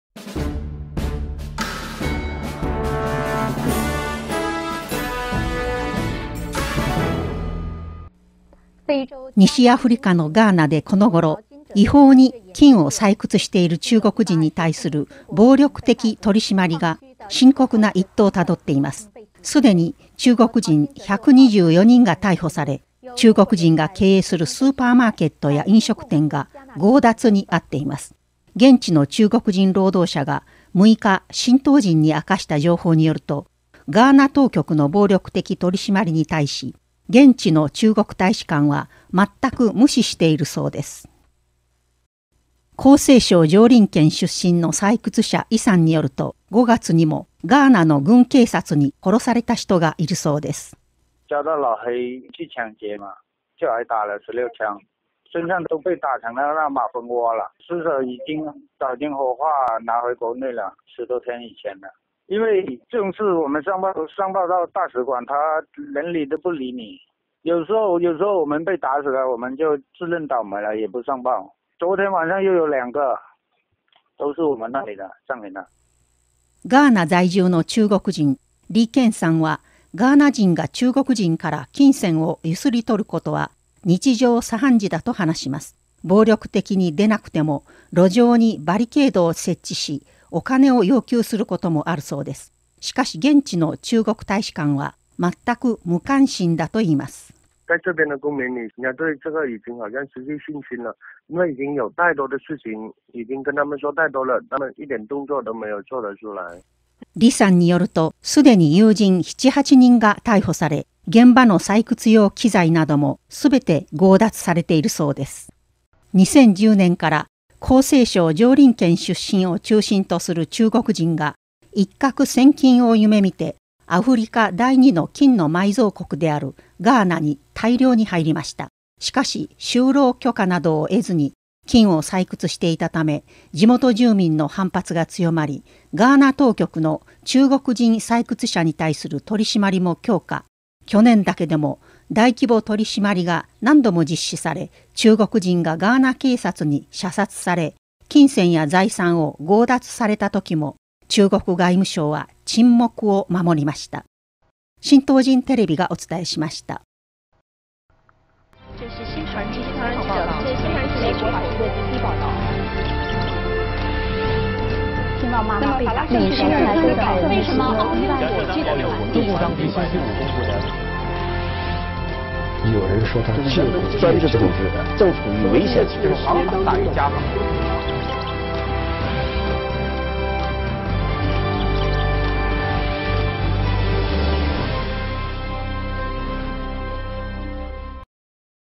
ガーナで中国人金採掘者虐殺 中国大使館は無関心
【新唐人2013年06月07日付ニュース】西アフリカのガーナでこのごろ、違法に金を採掘している中国人に対する暴力的取り締まりが深刻な一途をたどっています。すでに中国人１２４人が逮捕され、中国人が経営するスーパーマーケットや飲食店が強奪に遭っています。現地の中国人労働者が６日新唐人に明かした情報によると、ガーナ当局の暴力的取り締まりに対し、現地の中国大使館は全く無視しているそうです。